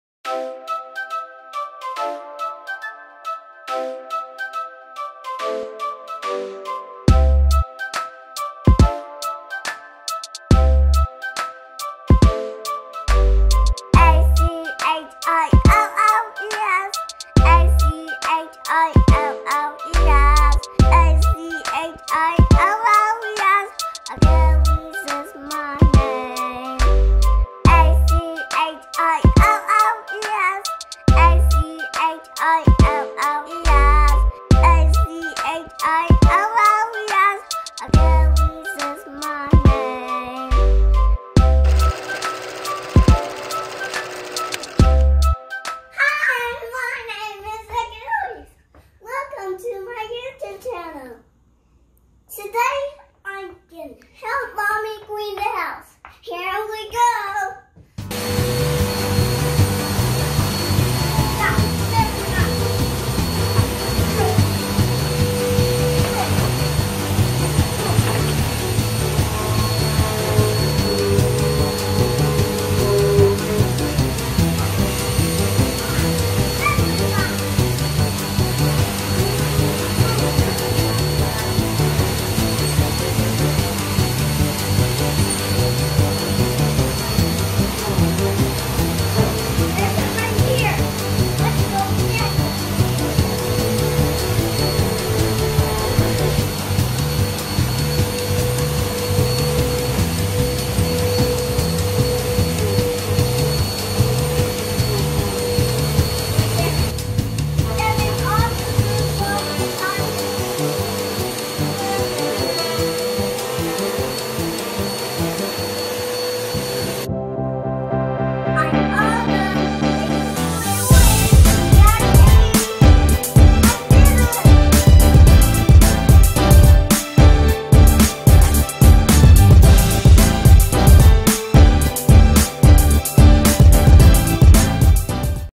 AchillesStory: Cleaning the House
Hi!

Welcome to AchillesStory YouTube channel!

Here, Achilles is being a great helper by vacuuming all the "germs" off the carpet. And although the vacuum cord gets a little tangled up, he doesn't give up and pushes through.

Follow AchillesStory on YouTube, Instagram and TikTok for more of his stories.